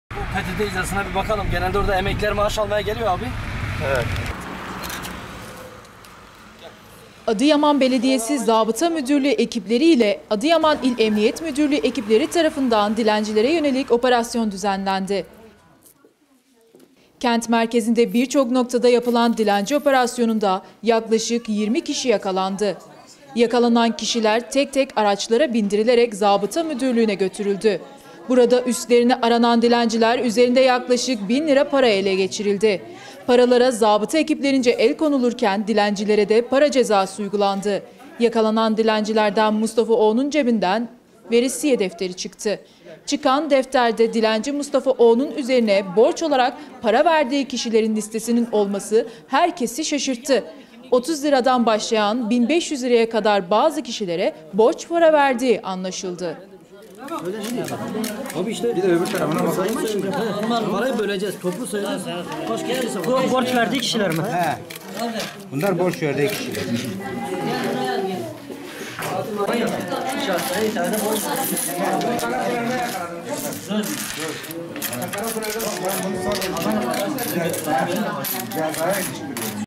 DİLENCİNİN CEBİNDEN ÇIKAN DEFTER HERKESİ ŞAŞIRTTI!
Yakalanan dilencilerden Mustafa O.'nun cebinden veresiye defteri çıktı. Çıkan defterde dilenci Mustafa O.'nu borç olarak para verdiği kişilerin listesinin olması herkesi şaşırttı. 30 TL'den başlayıp bin 500 TL'ye kadar bazı kişilere borç para verdiği anlaşıldı. Dilencinin cebinden çıkan liste şoke etti!